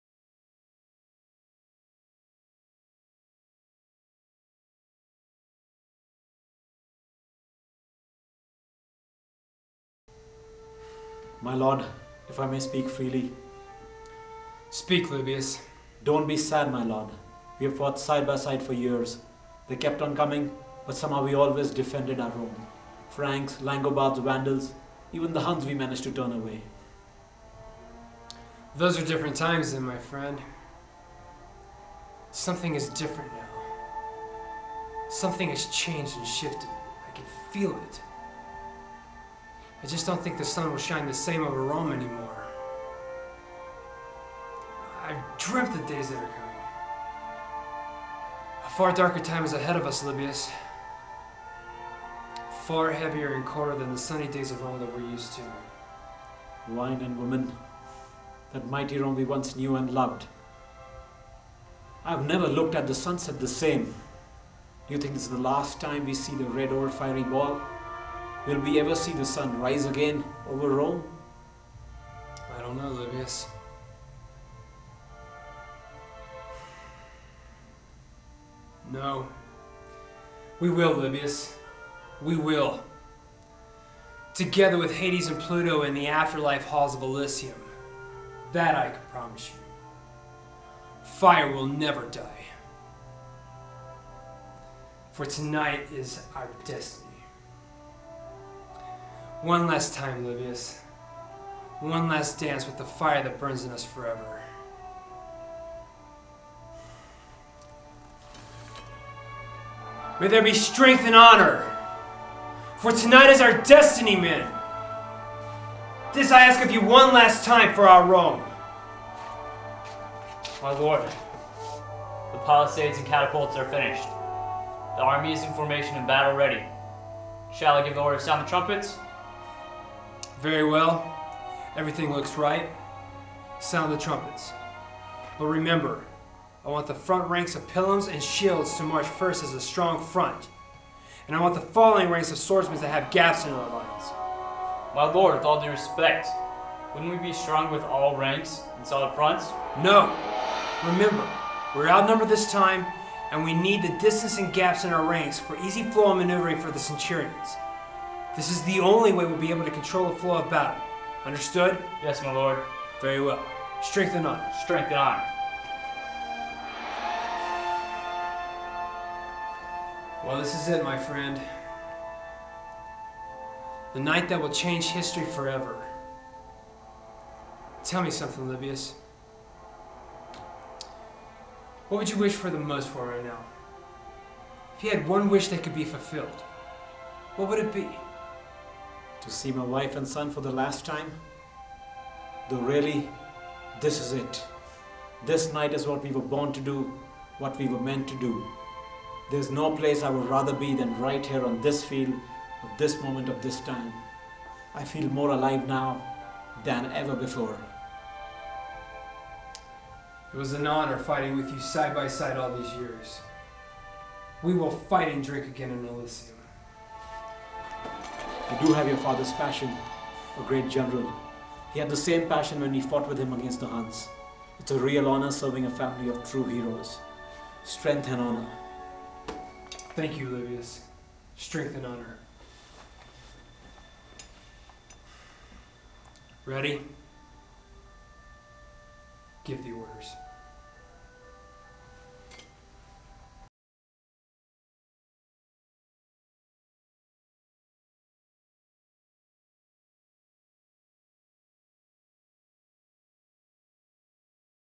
476 AD (PROMO) Test shoot auditions 2008.
This is a PROMO (Promotional footage) of one scene acted out in a theatre, from a screenplay I have written about the very end of Roman Empire, called "476 AD".
It is meant to show the feel, and the ambience of the story, to potential investors.

It is the eve of September 3, 476 A.D. The eve of The Ancient Age, and the donning of the New Dark Ages. ROME has existed for 12 centuries, and it is at it's very end.

As the young CAESAR ROMULUS AUGUSTULUS sits on the throne. The Germanic Barbarians OSTROGOTHS, under their leader ODOACAR, are at the gates of Rome, ready to crush it once and for all after centuries of wars.
Only one last stronghold remains, as they need to defeat the last of the best of Roman Centurions.

In this scene, Roman general AETIUS PATRIUS MAJORIAN, played by CHUCK STRICKLAND, is being comforted by his second in command, Centurion commander LIBIUS SEVERUS, played by ANUJ MEHTA.

As they wait for the sun to go down, right before the final last battle between the Romans and Germans is about to take place.

Ivan Pavletic

COPYRIGHT. ARTISK, INC. 2009.